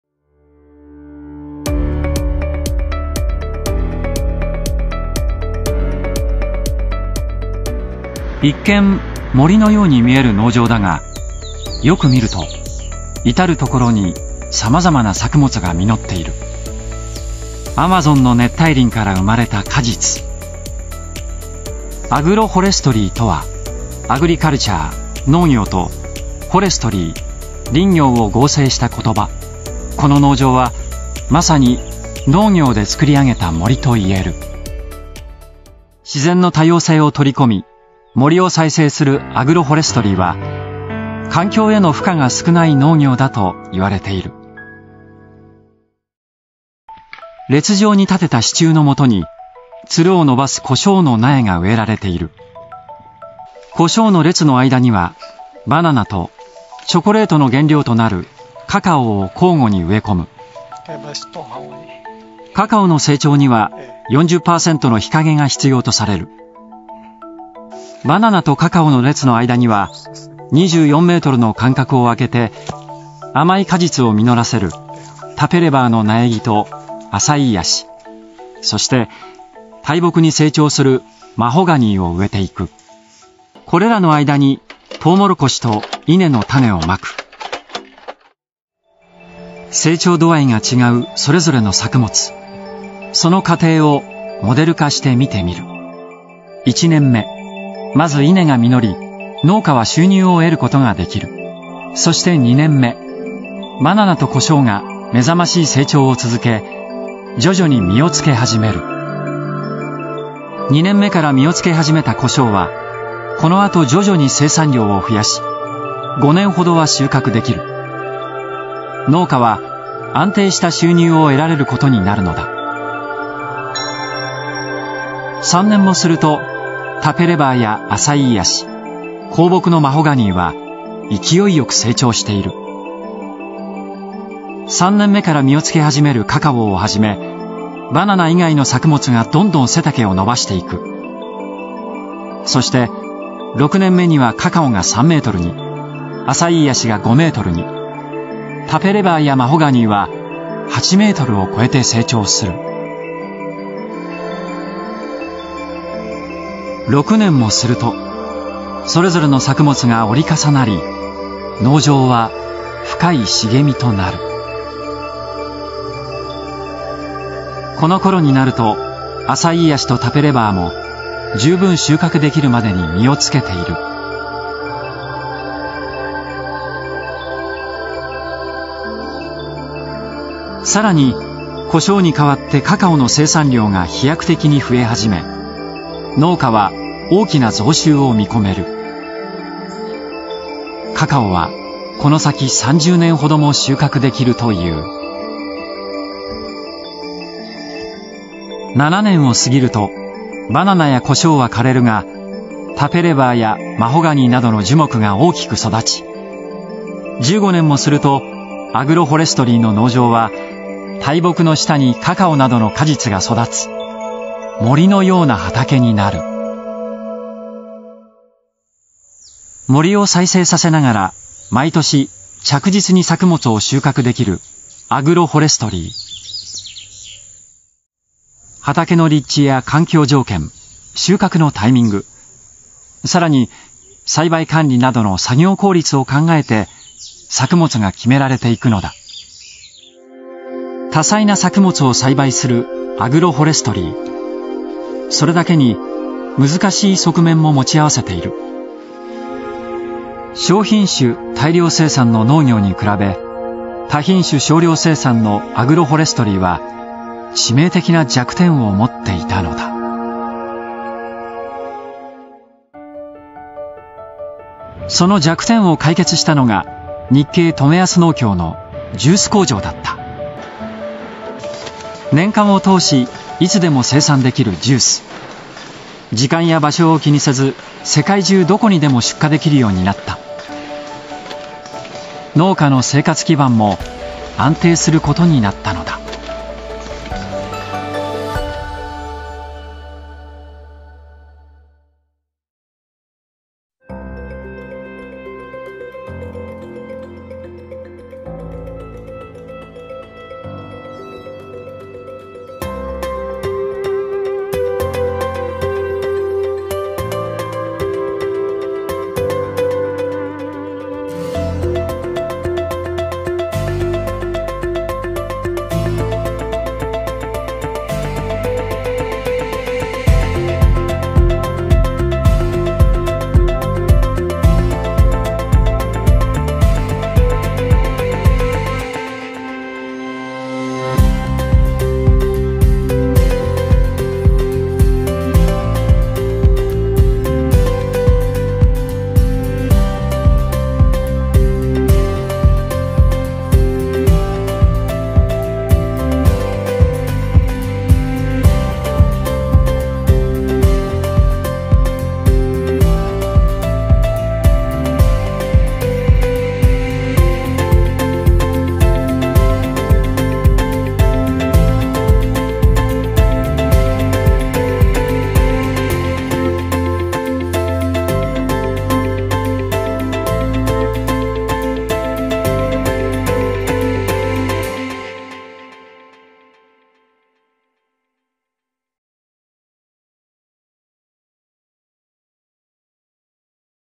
持続可能な農業「アグロフォレストリー」と、その実践者CAMTA農協 紹介動画
持続可能な農業として世界から注目される【アグロフォレストリー】とは、農作物と樹木を一緒に育てる農法の総称で、世界各地で様々な実践例があります。
もともとフルッタフルッタも20年ほど前からアグロフォレストリーの取り組みを支援してきましたが、経済・社会・環境の側面から多くのメリットがあるため、近年では世界中からサステナブルな農法として期待されています。農場内の植物の成長に伴ってCO2が吸収されていき、農地に炭素が固定されるため大気中のCO2削減につながります。
中でもブラジル・トメアスのアグロフォレストリーは、アマゾンの森林伐採された荒廃地に、多様な果樹や材木の苗と、その間に単年性の作物の苗や種を植え、森の様な農場を構成していきます。これにより1年目から収穫を得ることができ、従来の単一栽培ではできなかった持続的な生産を可能としています。
CAMTA（トメアス総合農業協同組合）は、ブラジルのアマゾン地域に位置するトメアスに渡った日本人移住者が1931年に創立した農業組合ですが、フルッタフルッタはCAMTAのアジア圏独占代理店であり、唯一無二のパートナー関係を築いています。

▼フルッタフルッタの最新情報は各SNSよりチェック！🦜